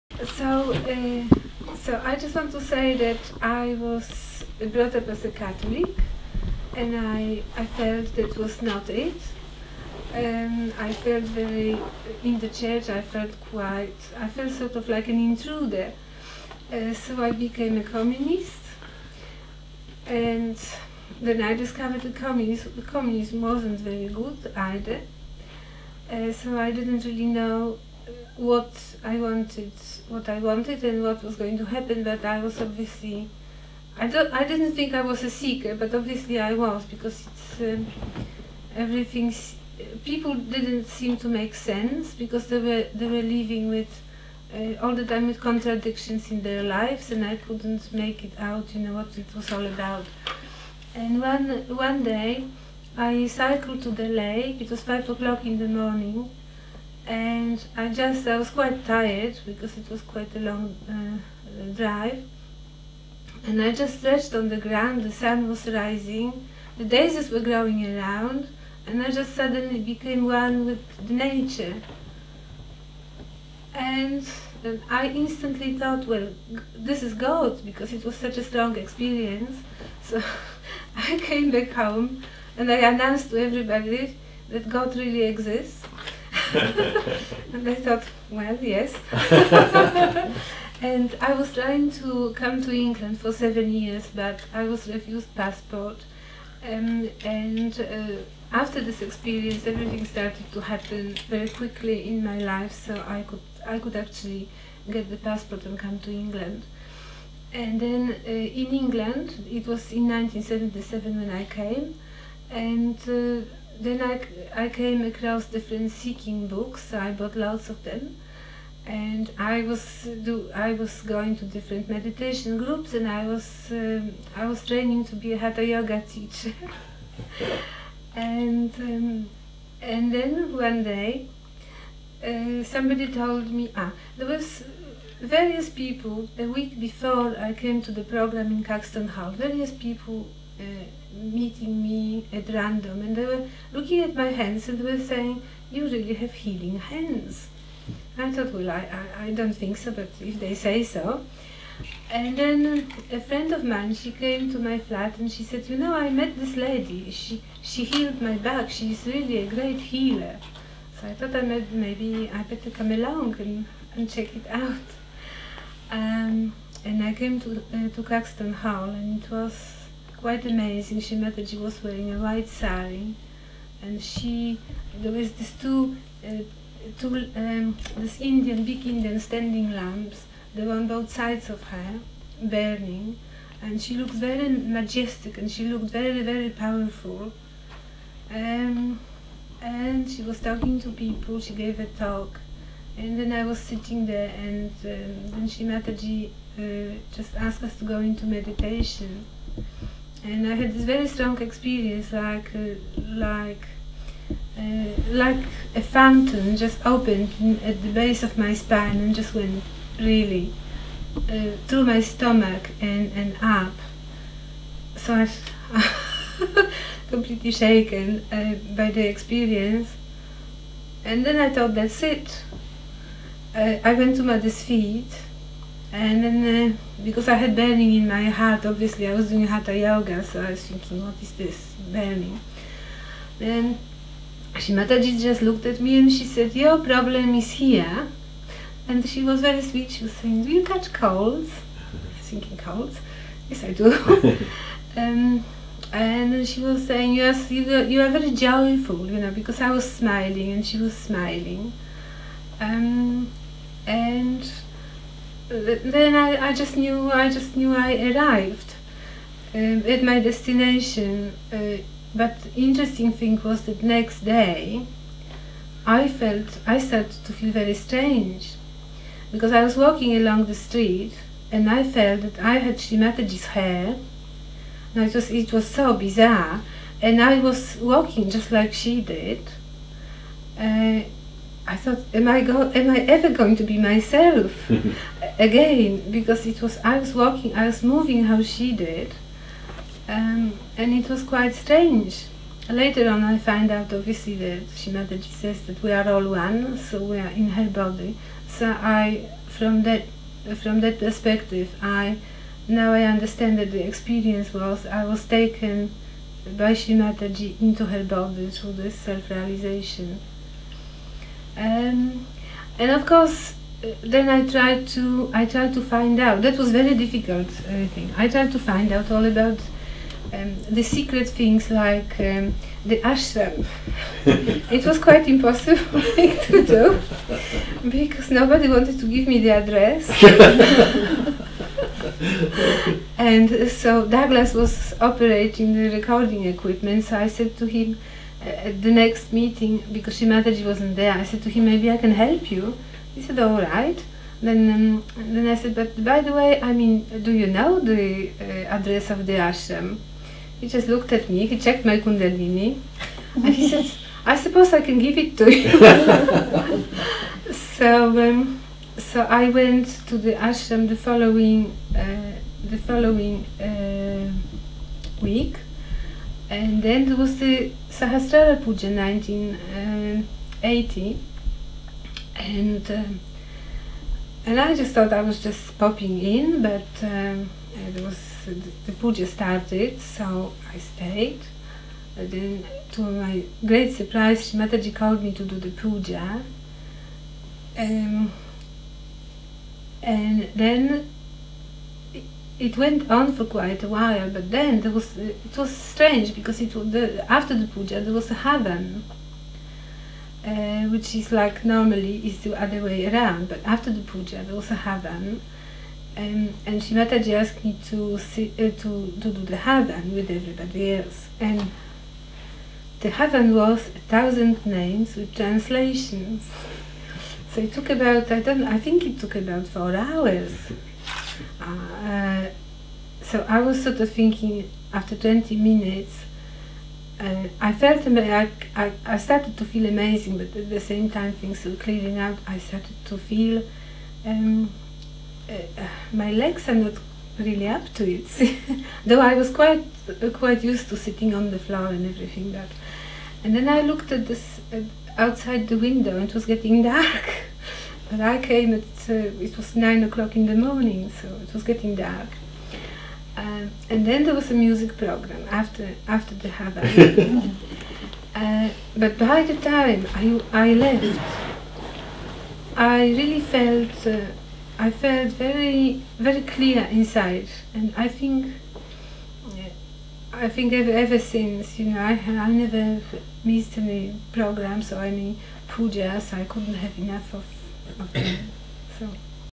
Grazyna Anslow, UK Golden Memories Sahaja Yoga
Sahaja Yogis remember their times spent with Her Holiness Shri Mataji Nirmala Devi. Grazyna Anslow